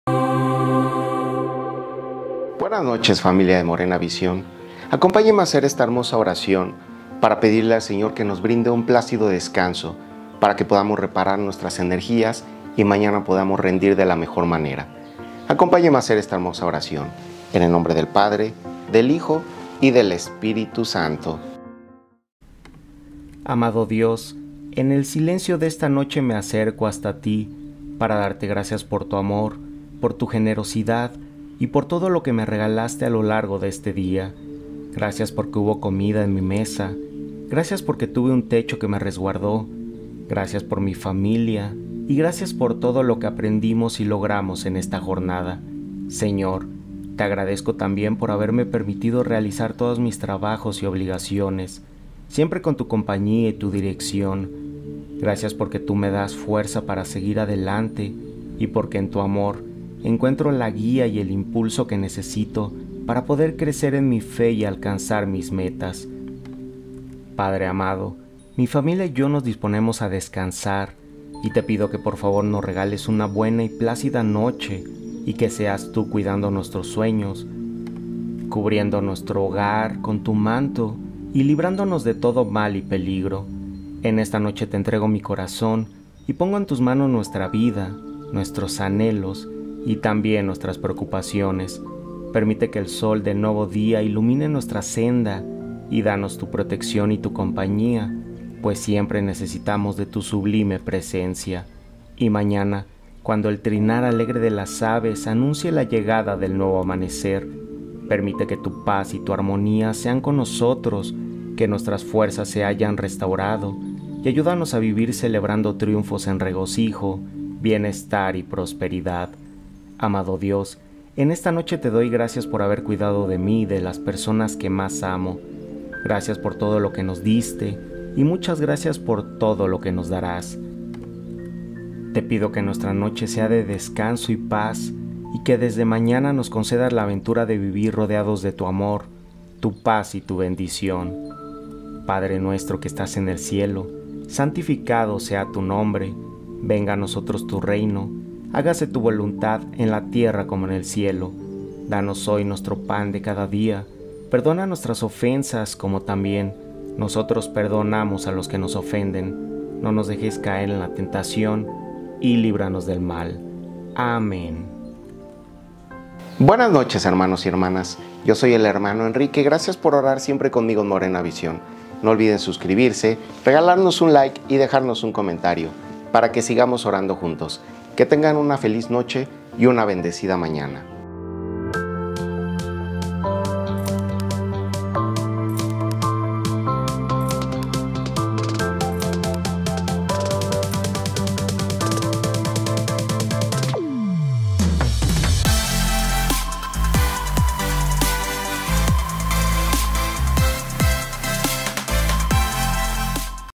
ORACIÓN PARA PEDIRLE AL SEÑOR UN PLÁCIDO DESCANSO [ ORACIONES DE LA NOCHE 🙏🏼 🌙 ] /MV ORACIONES
Oración para pedirle a Dios un plácido descanso - Padre amado, mi familia y yo nos disponemos a descansar y te pido que por favor nos regales una buena y plácida noche y que seas Tú cuidando nuestros sueños, cubriendo nuestro hogar con tu manto y librándonos de todo mal y peligro. Amén 🙏🏼

Oración de la noche de HOY Jueves 16 Junio 🙏🏼. Morena Vision Oraciones

En esta noche te entrego mi corazón y pongo en tus manos nuestra vida,
nuestros anhelos y también nuestras preocupaciones. Permite que el sol
del nuevo día ilumine nuestra senda y danos tu protección y tu compañía,
pues siempre necesitamos de tu sublime presencia 😇

Oraciones oraciones de la noche oraciones a Dios oraciones para dormir

MV. Oraciones